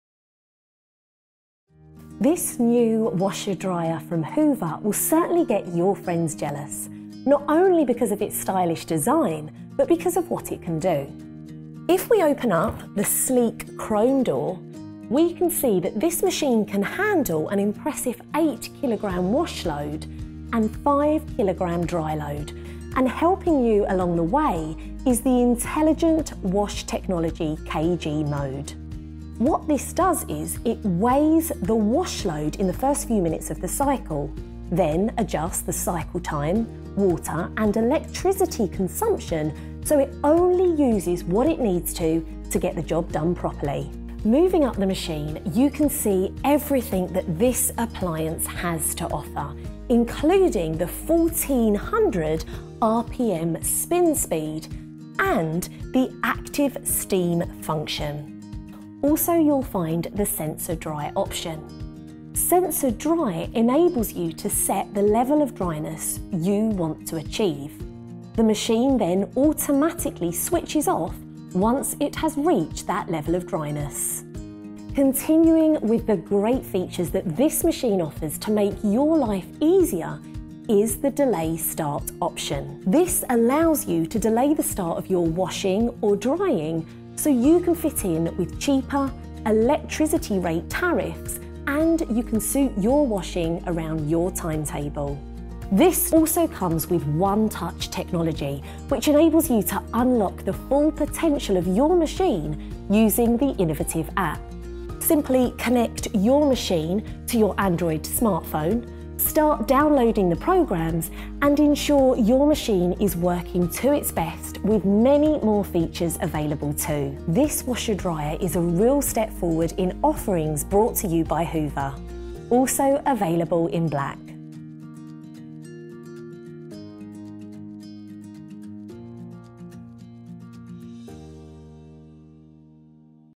Hoover Washer Dryer 8Kg/5Kg - WDXOA485C
59 minute wash and dry.

Sports wash.

Delay start.

Overview:

Condenser dryer.
8kg maximum washing capacity.
5kg maximum drying capacity.
12 washing programmes.
12 drying programmes.
Size H85, W60, D60cm.
Variable washing temperature.
Variable spin speed.
Washing programmes:

Cotton.
Delicates.
Automatic half load.
Hand wash.
Intensive.
Mixed load.
Pre-wash option.
Reduced creases.
Sensitive.
Silks.
Synthetics.
Wool.
Controls:

NFC enabled.
Dial control.
LED display.
Time remaining indicator.
Drying features:

3 heat settings.
180 minute timer.
Anti-crease option.
180 minute quick dry.
Sensor dry with 4 dryness levels.
Performance:

Energy efficiency rating: A+++.
Maximum spin speed 1400.
Estimated annual water consumption: 23000litres/year.
Washing performance rating: A.
Spinning performance rating: A.
Washing noise level 61dB(A).
Spinning noise level 79dB(A).
General features:
Cold water fill.
Pause facility.
70.5kg.